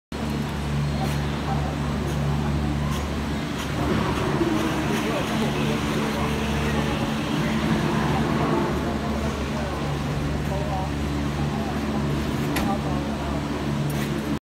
Genting Highlands -book hotel with addraa
Short weekend gateway ,

Enjoy skyway 360 view cable car at genting highlands malaysia
get the cheapest accommondation at genting highlands by addraa.